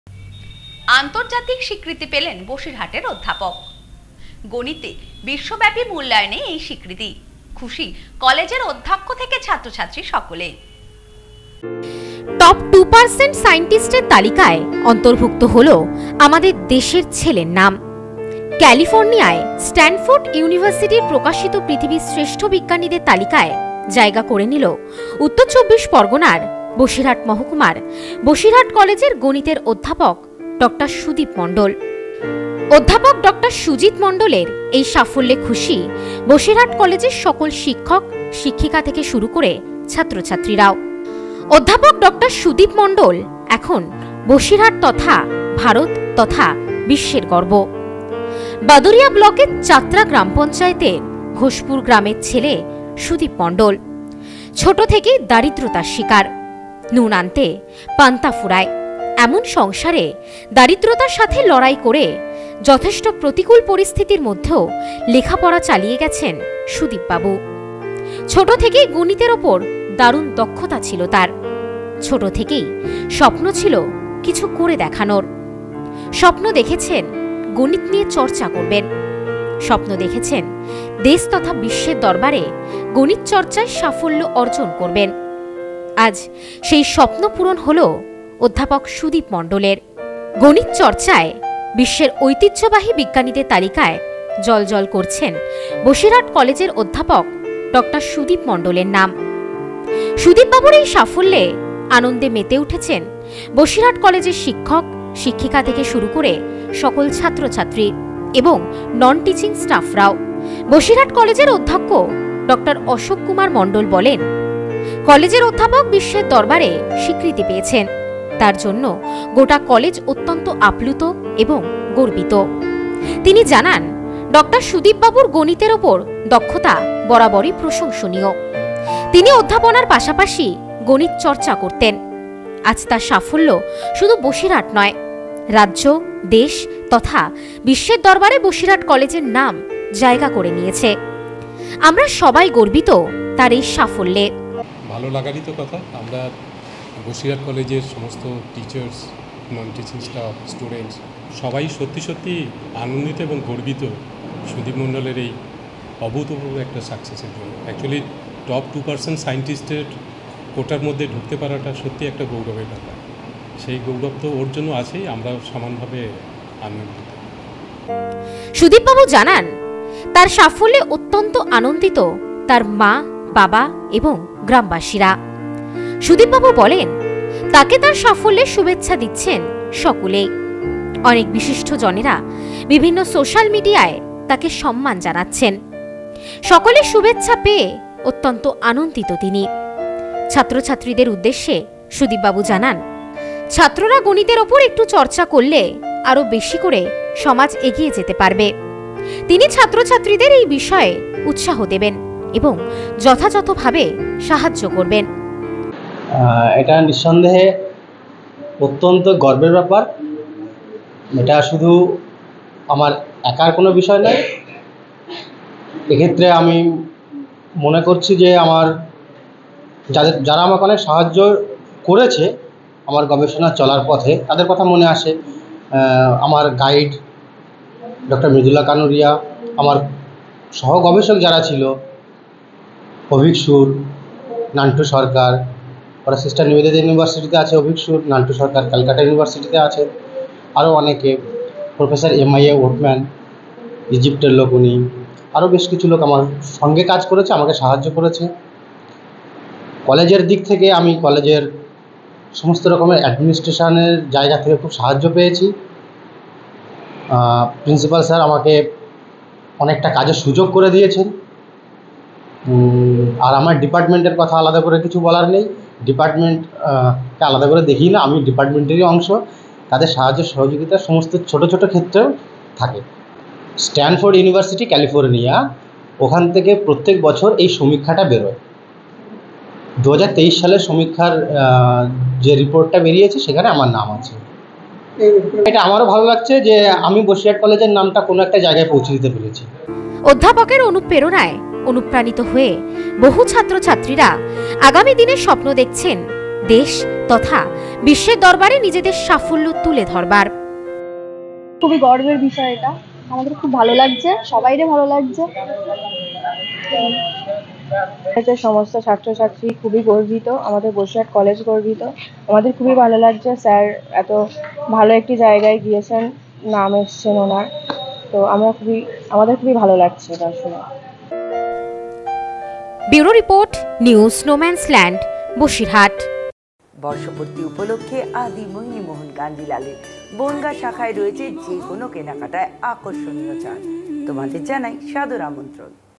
আন্তর্জাতিক স্বীকৃতি পেলেন বসিরহাটের অধ্যাপক।
আন্তর্জাতিক স্বীকৃতি পেলেন বসিরহাটের অধ্যাপক। গণিতে বিশ্বব্যাপী মূল্যায়নে এই স্বীকৃতি। খুশি কলেজের অধ্যক্ষ থেকে ছাত্র-ছাত্রী সকলে।